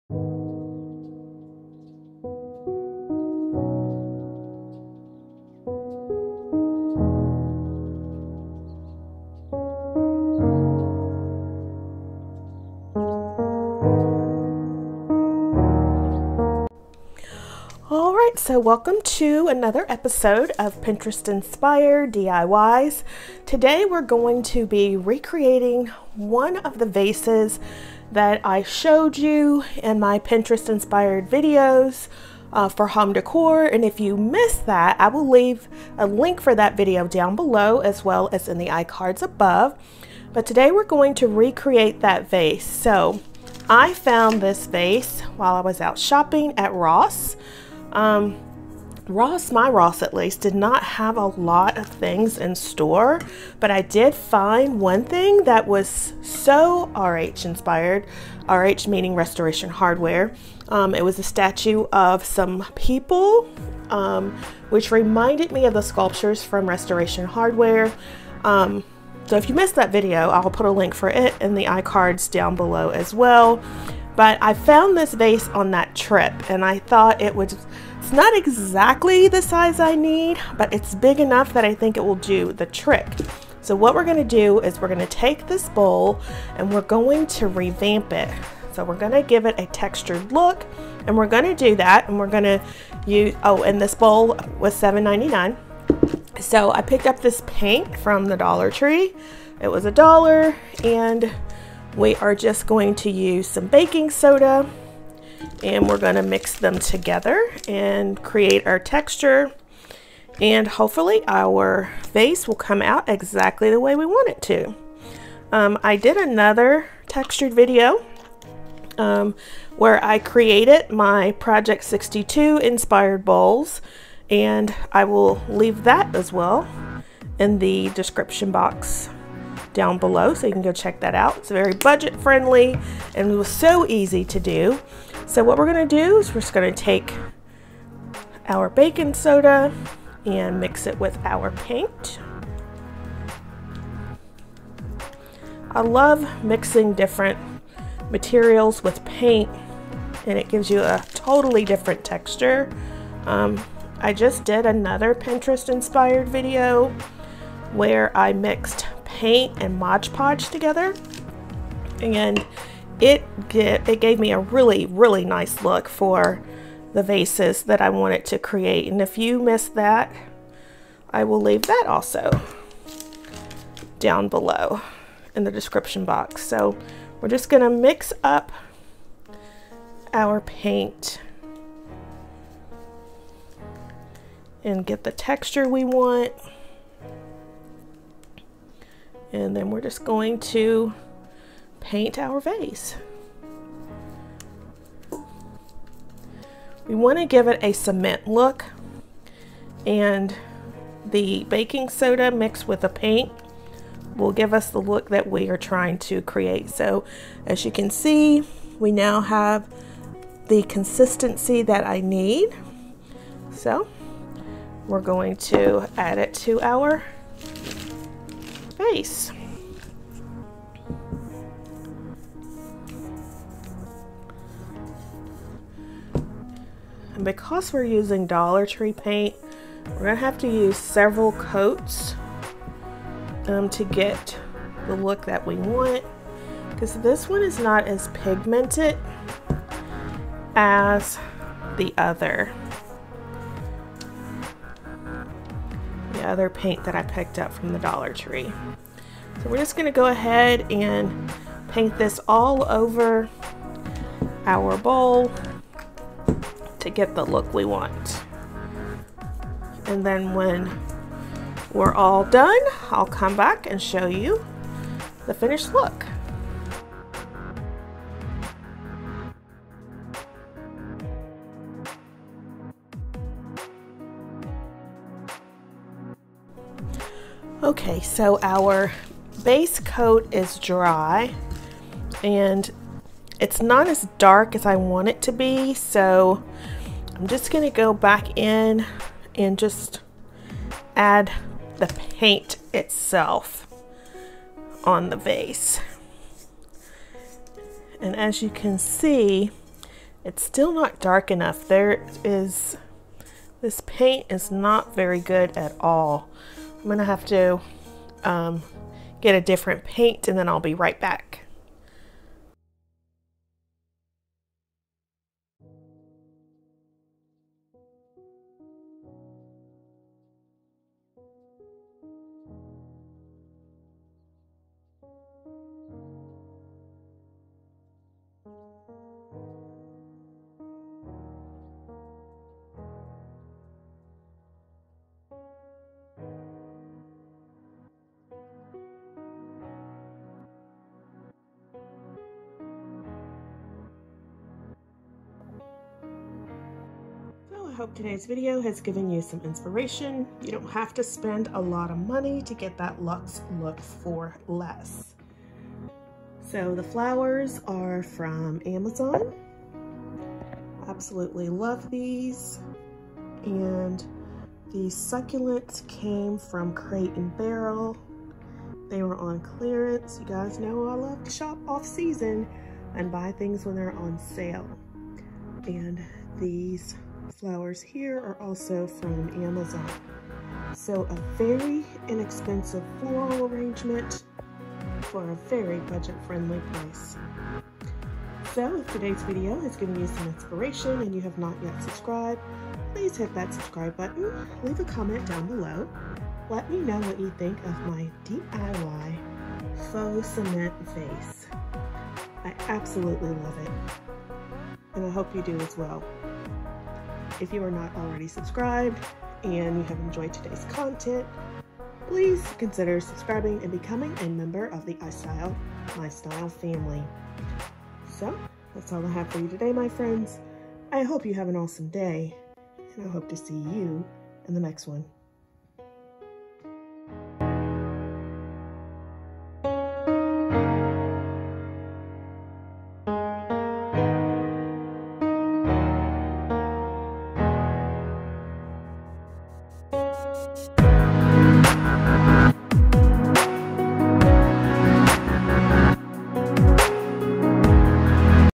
HOW TO DIY BUDGET FRIENDLY HIGH END HOME DECOR|PINTEREST INSPIRED HOME DECOR|LUXE FOR LESS
Hey iStylers!

In today's video I will show you how to create a budget friendly DIY high end home decor dupe.  I'm showing you how I created my Pinterest Inspired High End Home Decor DIY Dupe, giving me that luxe look for less.  It is very budget friendly and it gives you the high end look on a budget.

I hope you enjoy today's Pinterest Inspired Luxe Look for Less video.

What do you think of my budget friendly diy, do you think it looks high end?  Would you like to see how I create my floral arrangements to give you that luxe look for less?

TBUY 5 Pack Fake Artificial Flowers Lily for Home Hotel Decor

Duovlo Springs Flowers Artificial Silk Peony Bouquets Wedding Home Decoration,Pack of 1 (Spring White)

XoXo

Sheryl